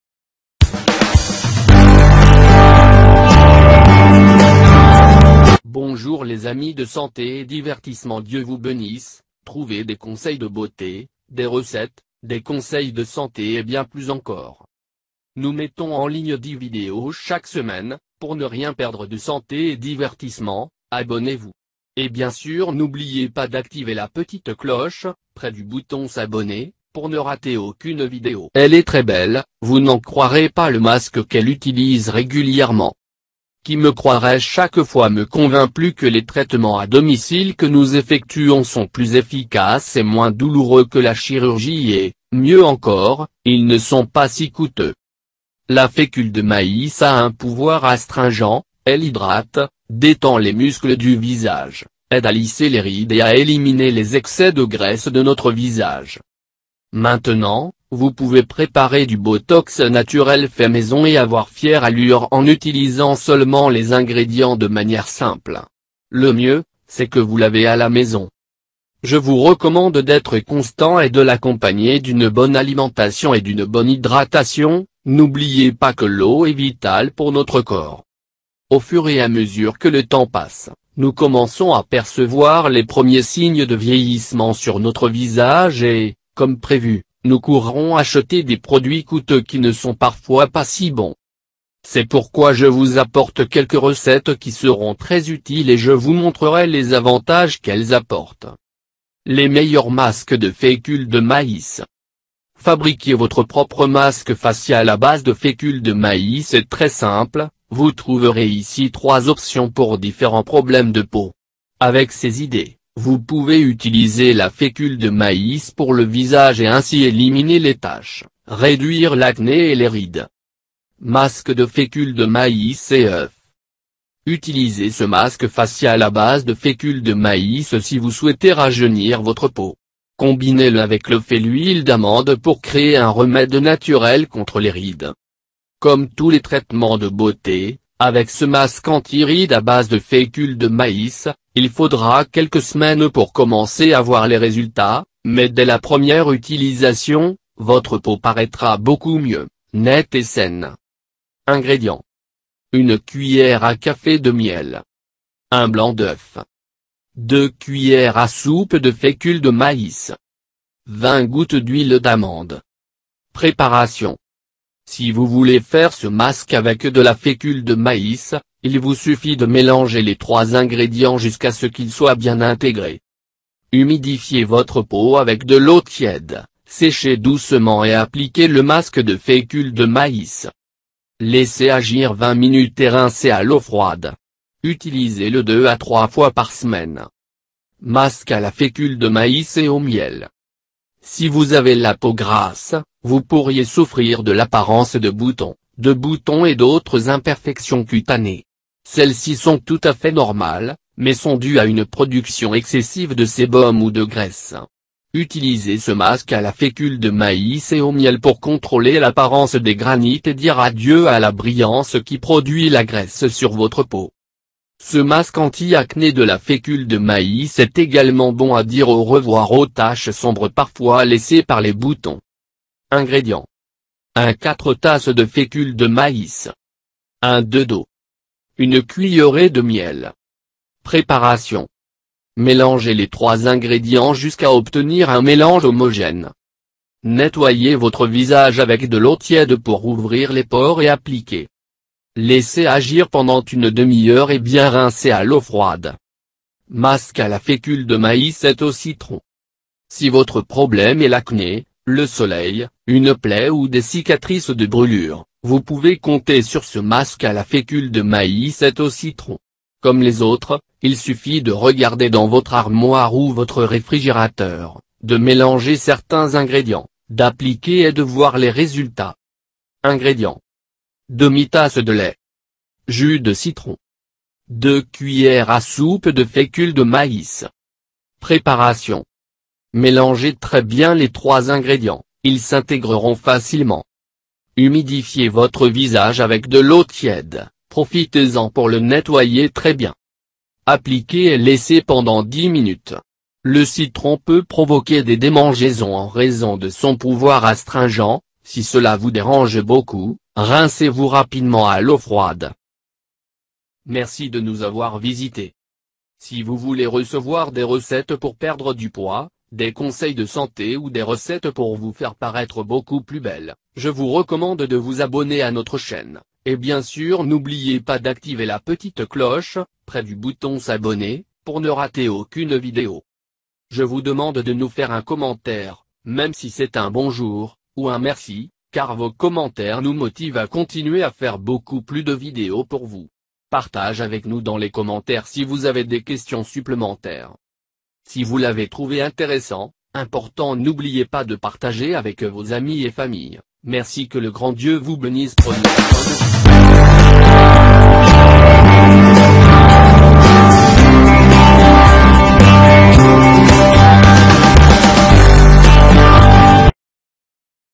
Elle a un beau visage, vous n'en croirez pas le masque qu'elle utilise régulièrement pour rajeunir
Autres masques cutanés
Il ne fait aucun doute qu'il existe un masque de fécule de maïs pour vous. Mais si vous voulez essayer d'autres options ou les combiner avec d'autres traitements de beauté naturels, vous avez ici des options de masques à ne pas manquer.

Pour peau sèche. Avec l'aide d'avocat, d'oeuf et d'huile d'amande, votre peau cessera de paraître sèche et vous retrouverez son aspect naturel et jeune.
Pour les peaux matures. Si vous avez aimé la fécule de maïs ou la crème de fécule de maïs pour les rides, vous ne pouvez pas arrêter d'en essayer une autre à base de jus d'orange, de camomille et de miel avec des effets très similaires.
Pour une peau normale. On parle toujours de crèmes et de masques pour les peaux sèches ou grasses, mais qu'en est-il de la normale? Si vous avez ce type de peau, vous avez également des options à portée de main avec des ingrédients tels que le fenouil et le miel.
REGARDER AUSSI:
Ce soin éclaircit votre peau en 7jours,vous n’allez plus jamais acheter de crème de soin Commentez👇